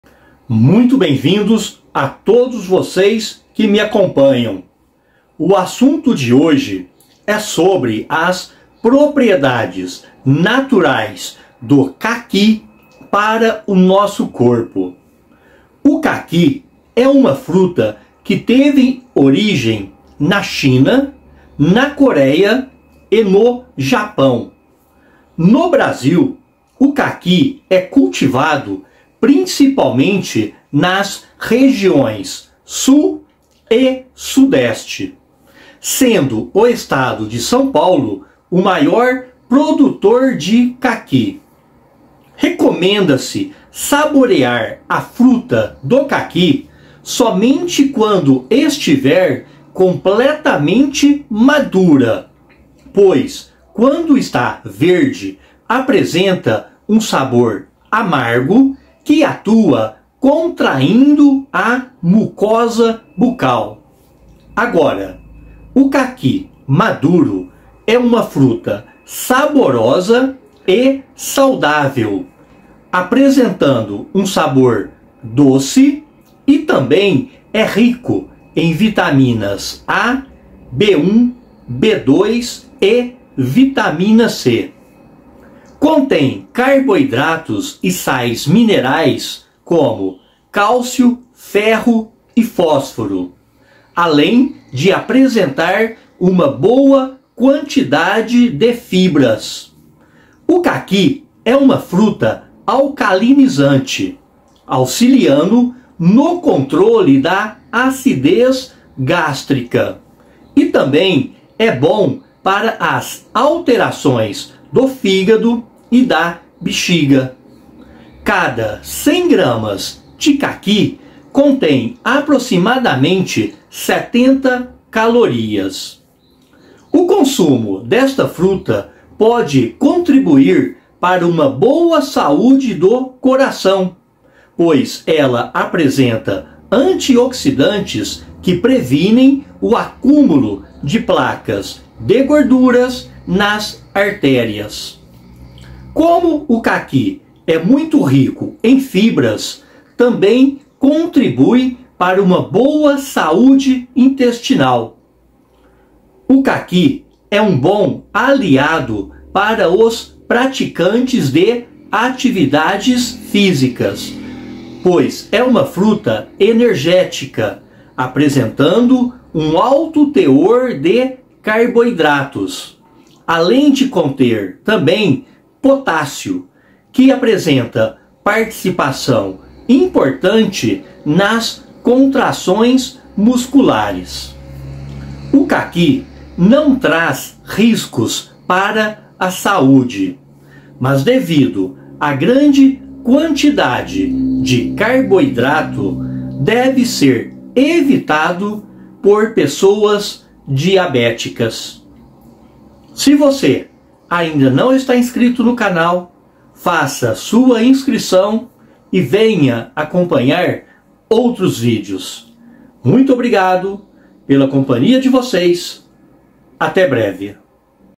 A saúde do caqui.
O caqui é uma fruta que teve origem na China, na Coréia e no Japão.
No Brasil o caqui é cultivado principalmente nas regiões Sul e Sudeste, sendo o estado de São Paulo o maior produtor de caqui.
O caqui maduro é uma fruta saudável, saborosa e de sabor doce. É rico em Vitaminas A, B1, B2 e C.
Contém carboidratos e sais minerais como cálcio, ferro e fósforo, além de ser rico em fibras.
O caqui é uma fruta alcalinizante, auxiliando no controle da acidez gástrica e também é bom para as alterações do fígado e da bexiga.